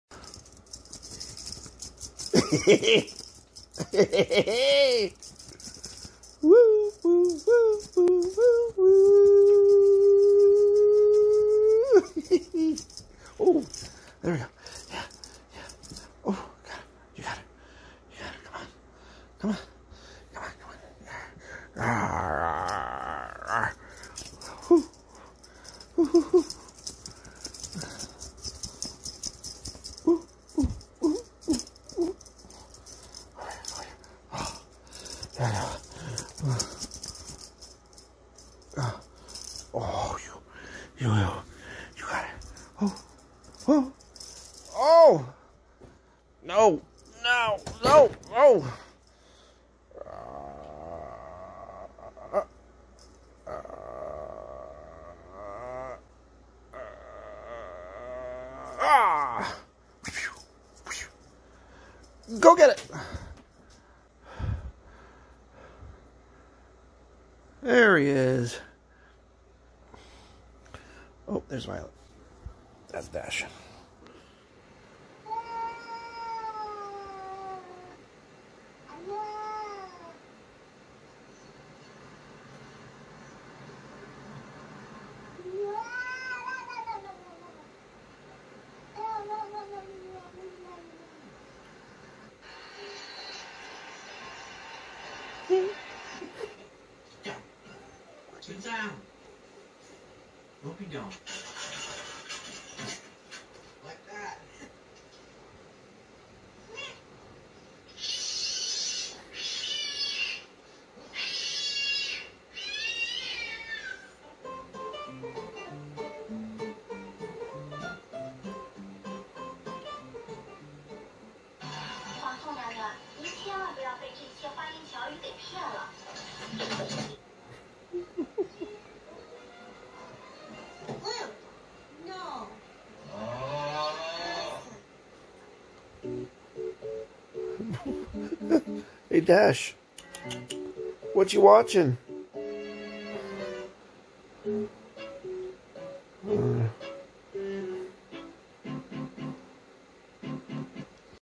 1,411 - Dash the Wonder Cat
Dash (aka Dashius) loves to play with his toy, and he also enjoys watching cat videos. ...

Are You a Dork? Follow THE SOCIALLY AWKWARD DORK on FACEBOOK